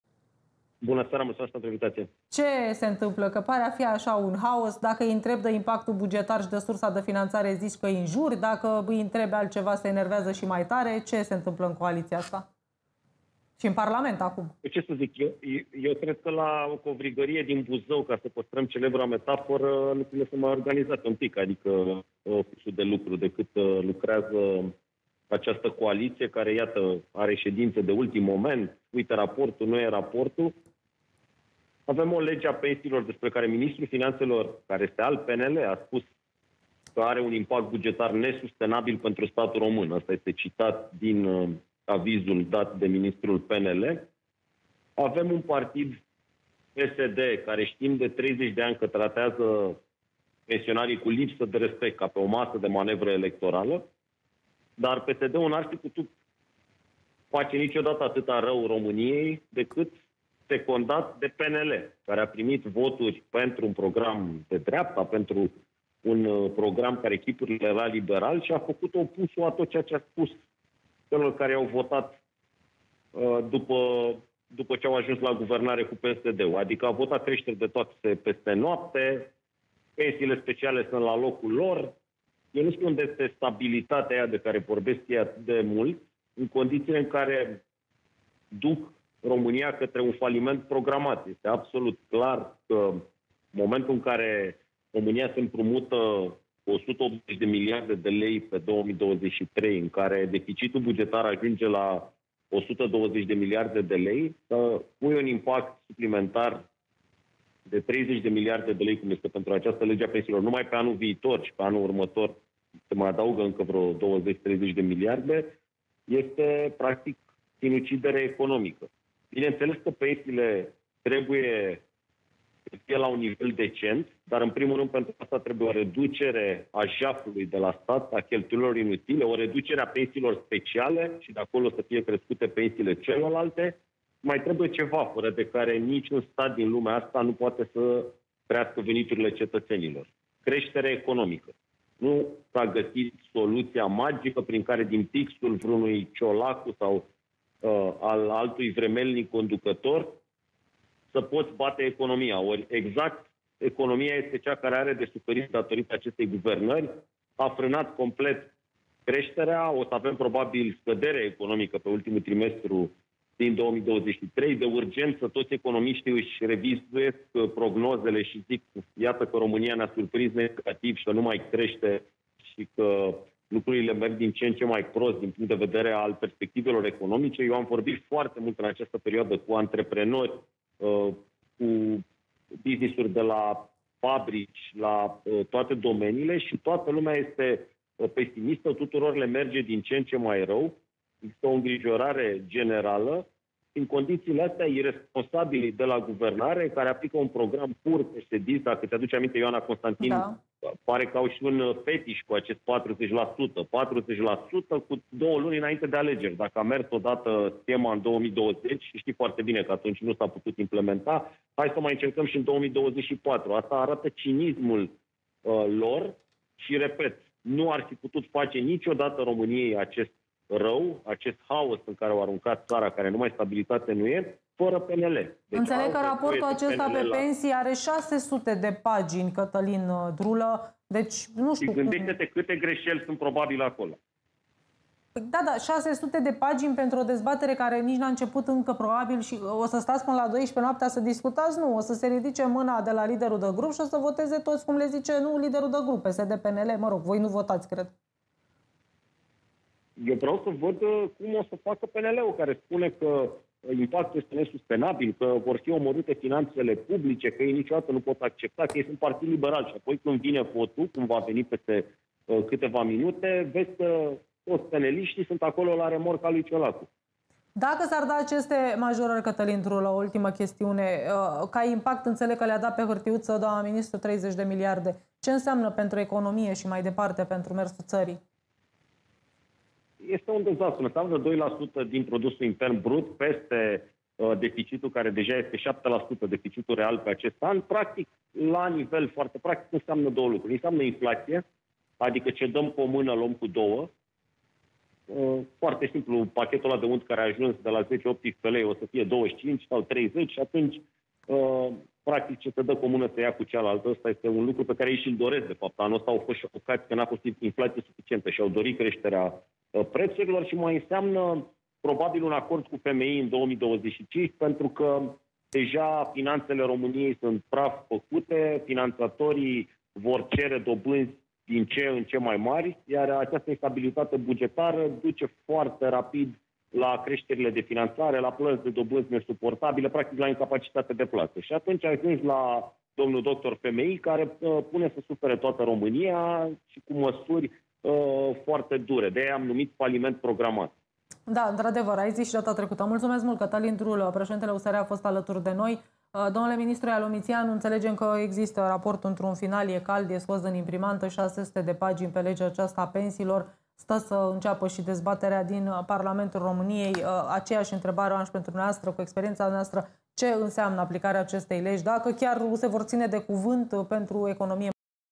Cătălin Drulă:„PSD și PNL duc România către un faliment programat/PSD nu ar fi putut face niciodată
atâta rău României, decât secondat de PNL”.